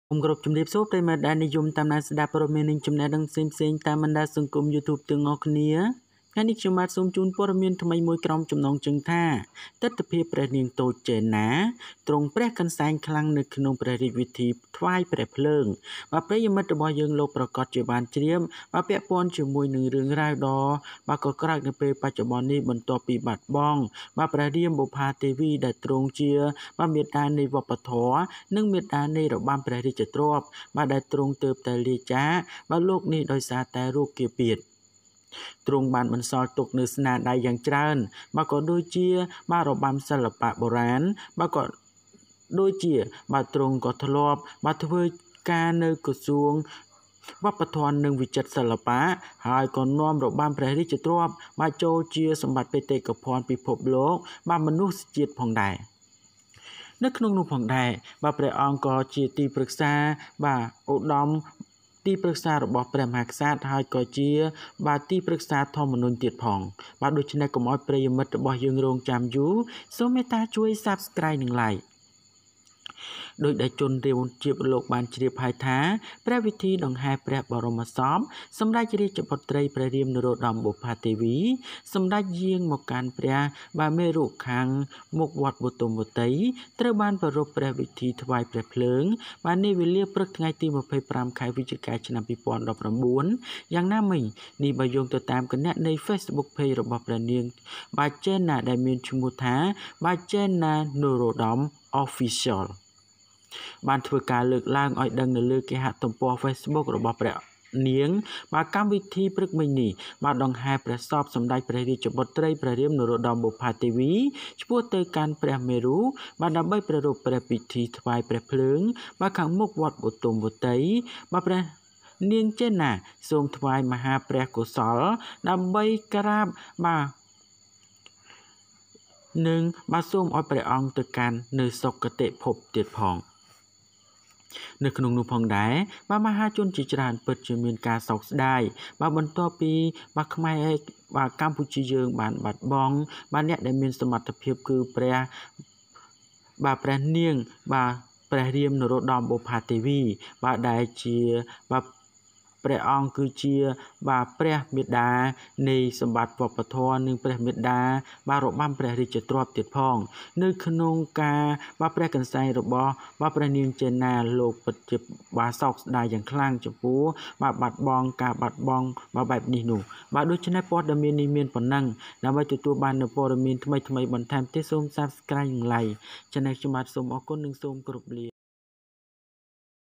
ទិដ្ឋភាពព្រះនាង ជេនណា ទ្រង់ព្រះកន្សែងខ្លាំងនៅក្នុងព្រះពិធីថ្វាយព្រះភ្លើង
ទិដ្ឋភាពព្រះនាង ជេនណា ទ្រង់ព្រះកន្សែងខ្លាំងនៅក្នុងព្រះពិធីថ្វាយព្រះភ្លើង The view of the princess Jena wept in the cremation.